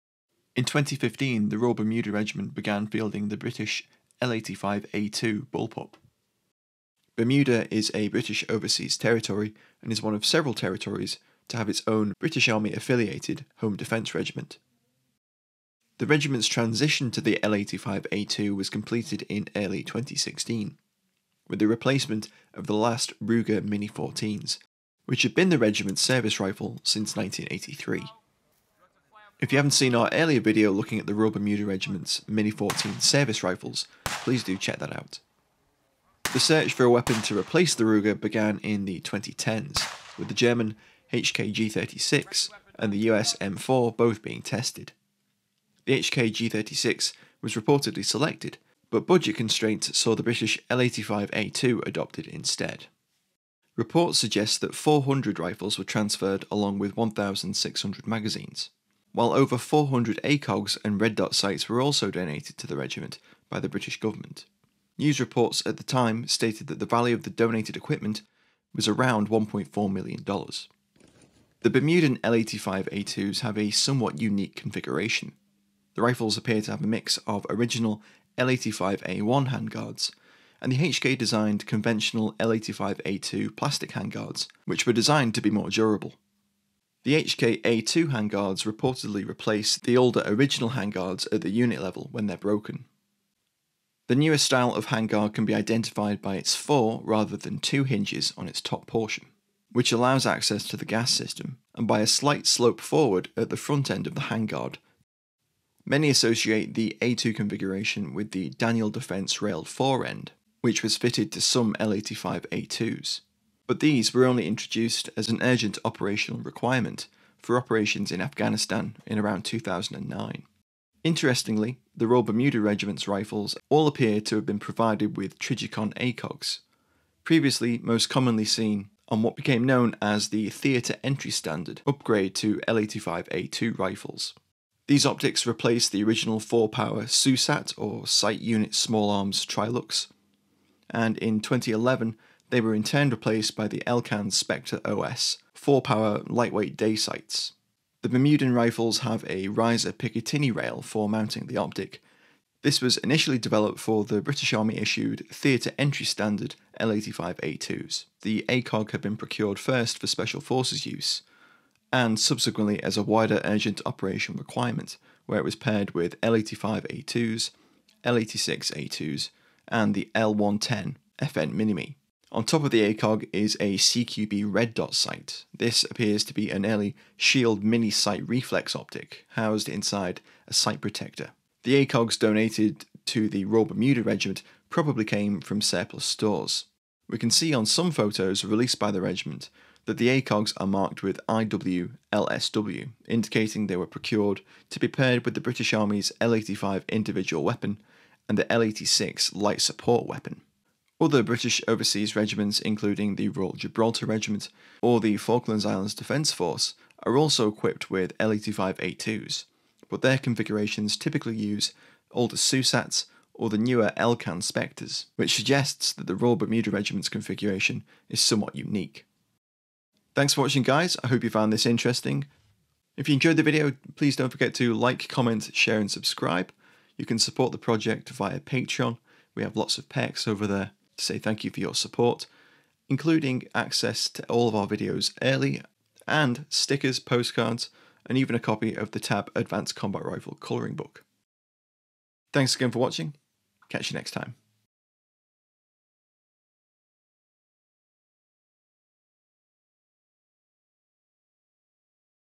The Rifle That Replaced Bermuda's Ruger Mini-14s
The Royal Bermuda Regiment utilises one of the most unique SA80/L85A2 configurations around. Combining a Trijicon ACOG originally purchased under an urgent operational requirement for Afghanistan and HK’s redesigned handguard.